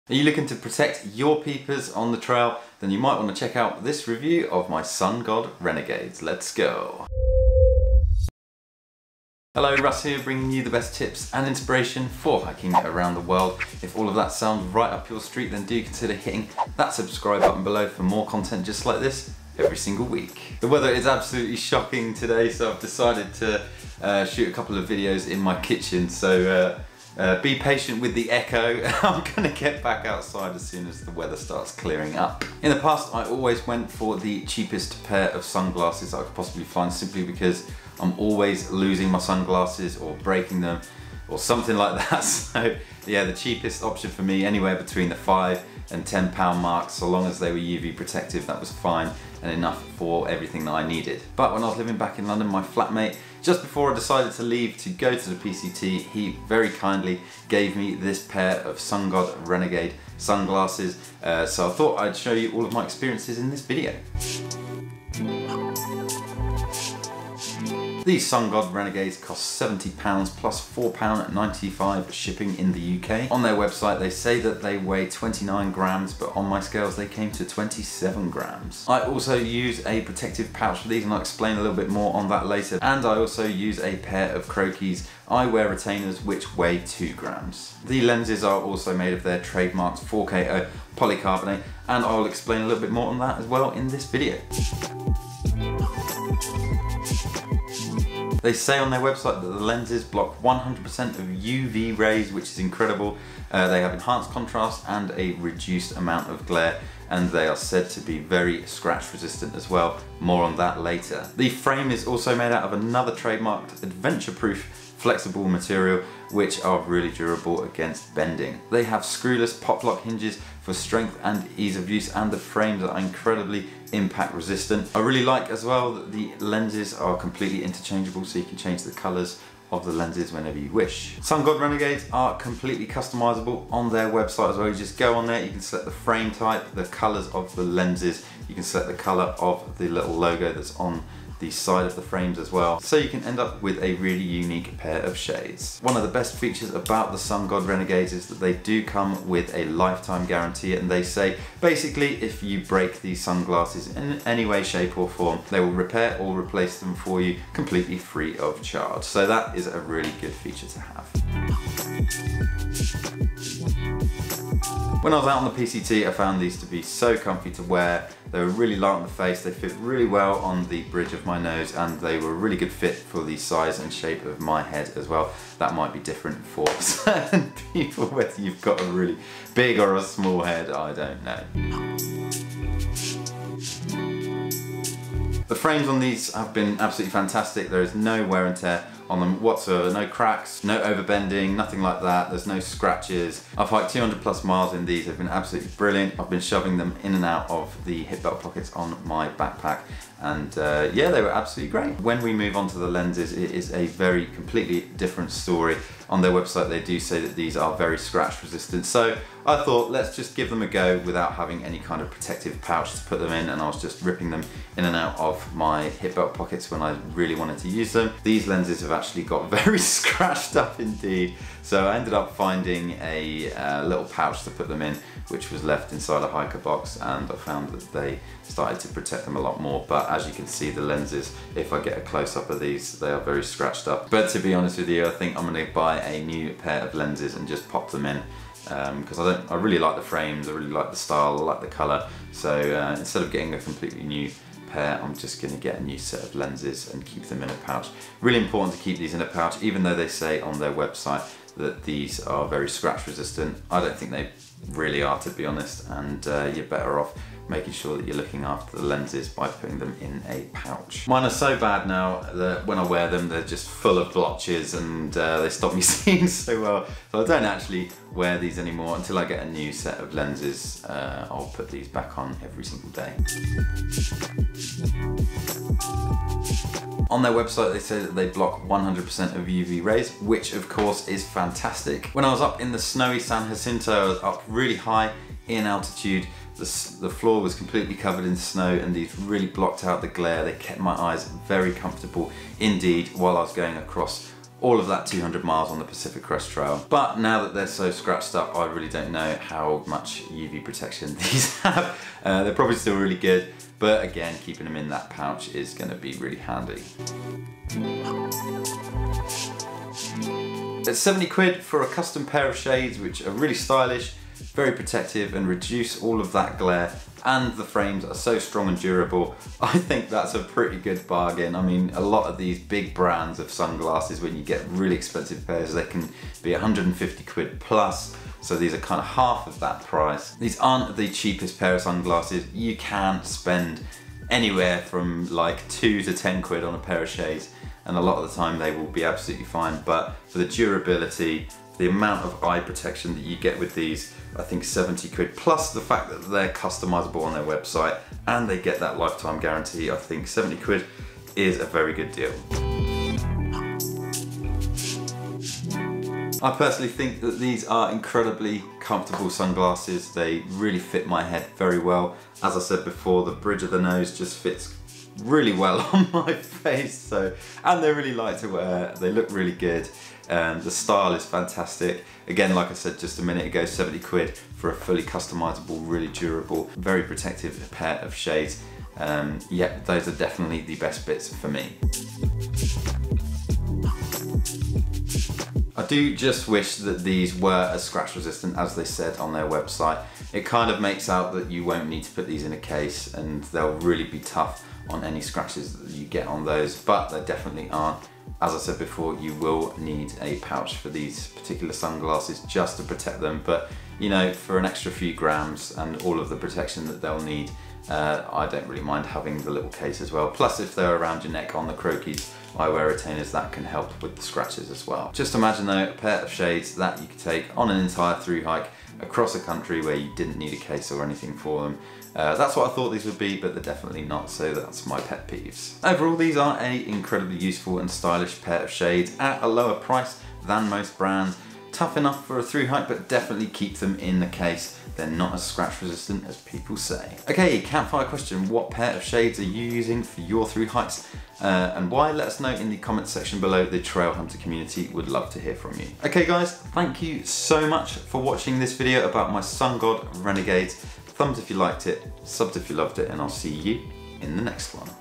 Sungod Renegade 2020 review | Best sunglasses for hiking?
Stylish, customisable and inexpensive! The Sungod Renegades were my sunglasses of choice for the Pacific Crest Trail. I've hiked more than 200 miles using them in some really bright sunshine on the PCT. They're durable, lightweight and not as expensive as some of the big brands!

They're the best sunglasses I've worn for hiking due to their durability in the frames, although the lenses aren't as scratch resistant as I'd hoped by what they say on their website, so definitely use the included pouch or a case to protect the lenses.

CLOTHING

FILM AND SOUND

NAVIGATION

WATER TREATMENT

ELECTRONICS

EQUIPMENT

STORAGE